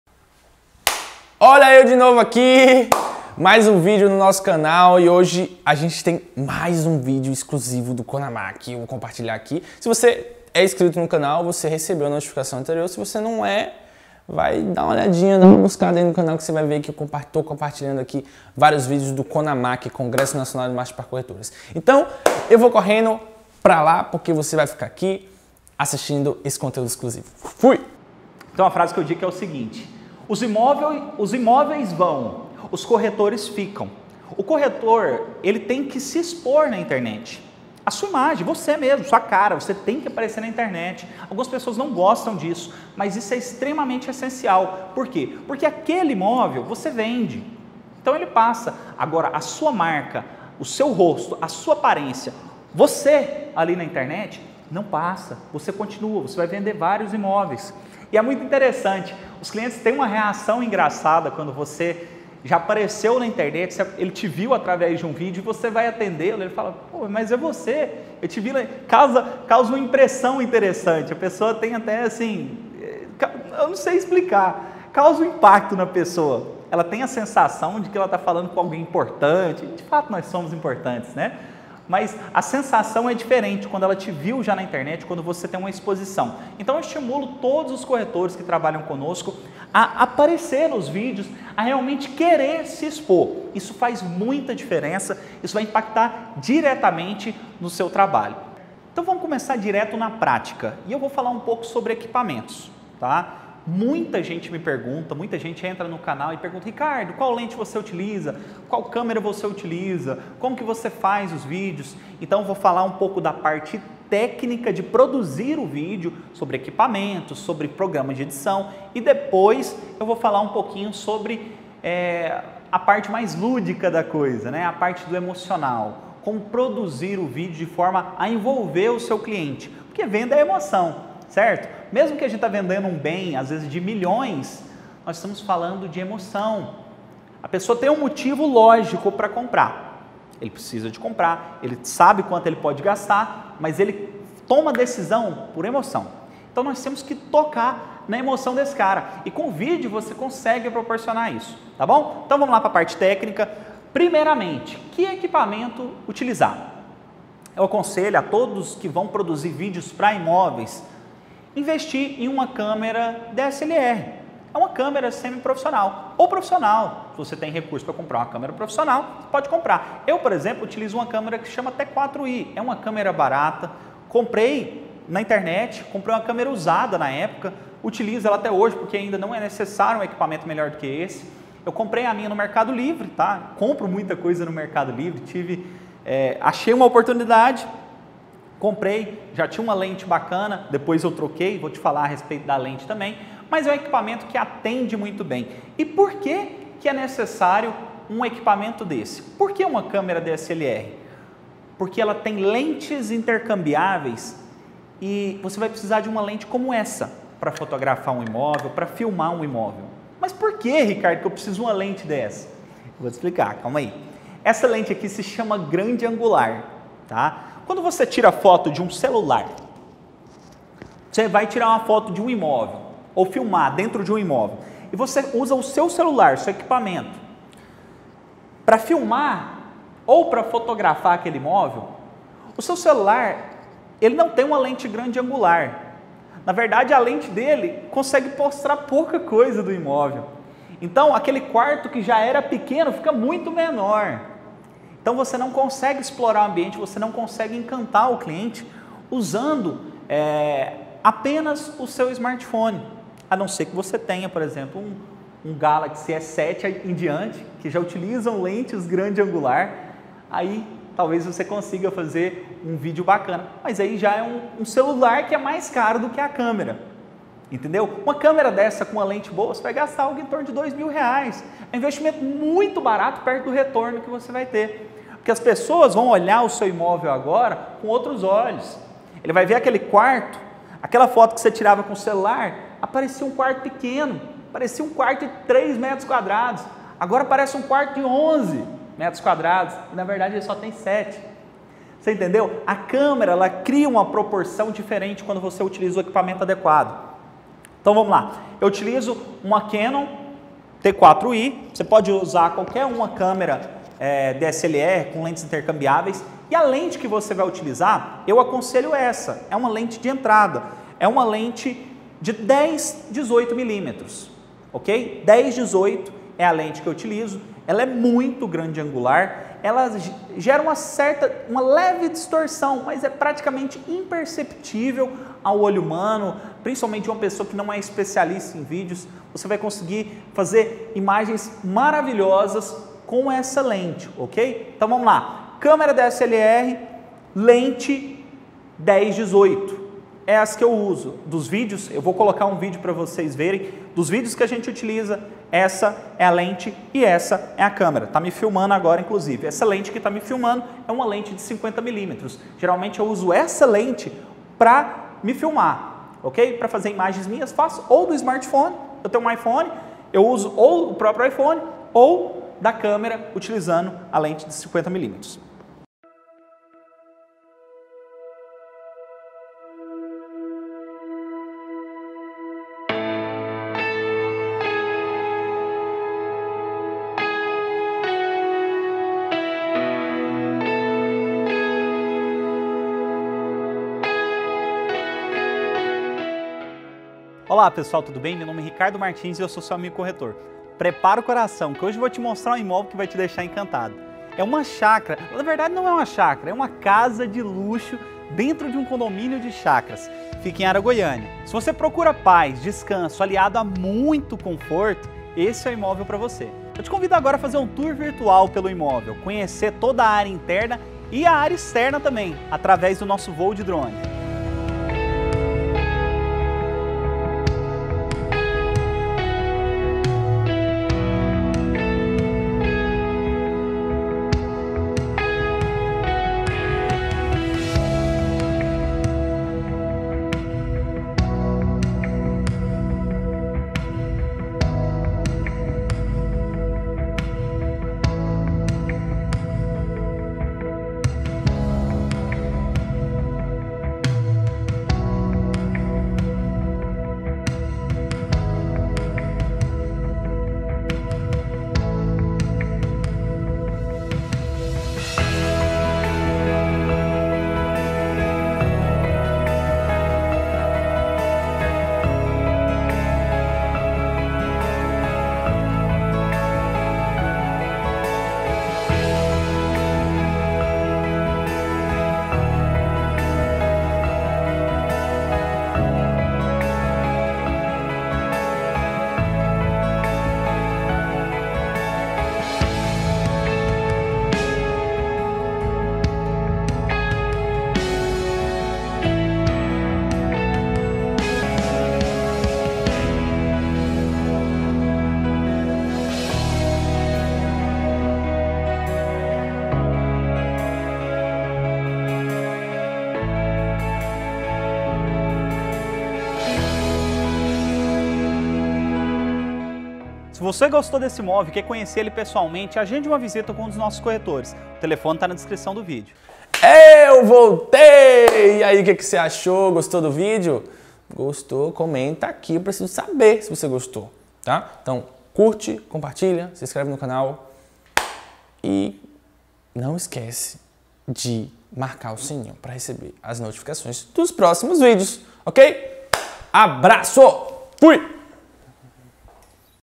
DE 0 Á 47 MIL INSCRITOS NO YOUTUBE - Ricardo Martins
Palestra do CONAMAC de Ricardo Martins

Especialista e apaixonado em produção de vídeos para imóveis, professor de Marketing Imobiliário no Centro de ensino tecnológico de Goiás, produtor do canal imobiliário Ricardo Martins no Youtube, sócio Fundador na empresa Amigo Corretor e Diretor Comercial na My Broker Imobiliária em Goiânia.

Palestra: O poder dos vídeos para fechamento de vendas e posicionamento no mercado

COMO ELE CONSEGUIU CHEGAR A 47 MIL INSCRITOS NO SEU CANAL FAZENDO O PASSO A PASSO QUE VAMOS COMPARTILHAR NESSA PALESTRA COM VOCÊ.

Tenha acesso ao Curso de Vídeo para Corretores  + Palestra completo do Ricardo Martins + CONAMAC 2016  + CONAMAC 2017